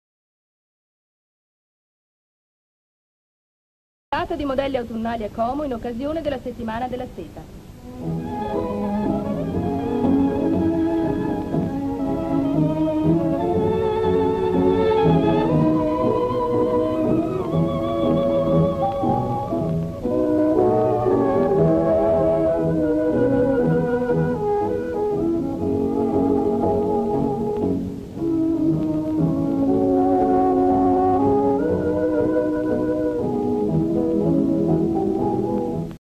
Sfilata di modelli autunnali a Como in occasione della Settimana della seta.
Descrizione sequenze:Modelle sfilano tra il pubblico seduto all'esterno presentando modelli autunnali ;